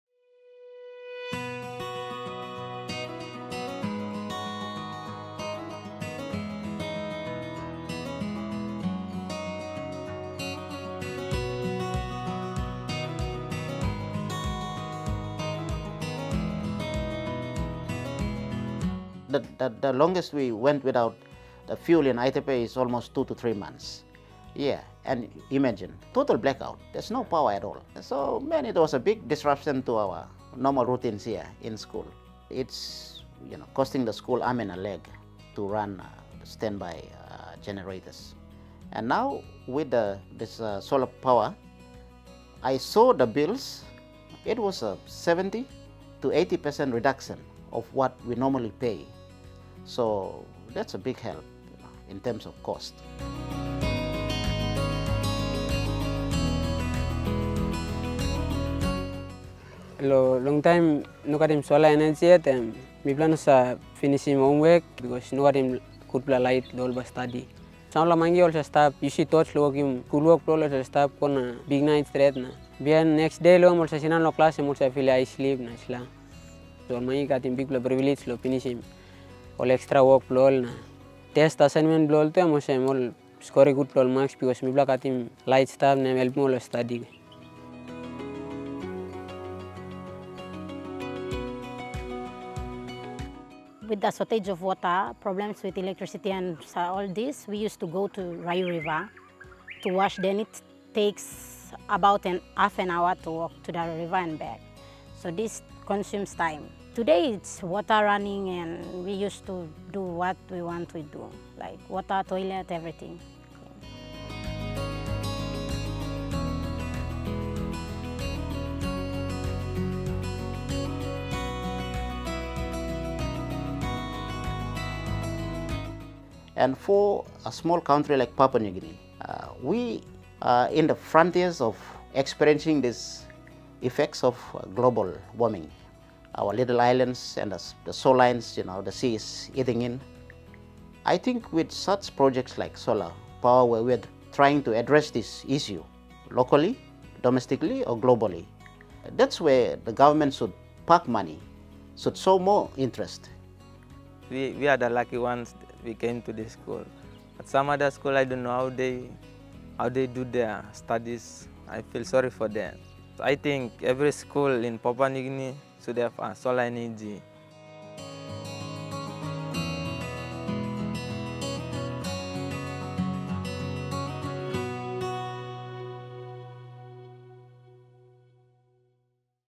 "Imagine a total blackout for 3 months": UNDP electrifies St. Ignatius Secondary School in Aitape
For years, St. Ignatius Secondary School located in the remote town of Aitape, Sandaun Province suffered from constant power blackouts. At worst, the school had no electricity for 3 months.

This caused major issues for the school's over 850 students, 85% of whom live on the campus as boarding students. Students completed homework with torches, water had to be fetched from a river 30 minutes away, and extracurricular activities were repeatedly cancelled.

In 2024, UNDP installed a solar energy system on the school grounds through the EU-funded STREIT PNG programme. Ever since then, the school has enjoyed a reliable supply of clean, renewable energy around the clock.

Watch this video to see how the solar energy has improved the life and learning environment of both students and faculty.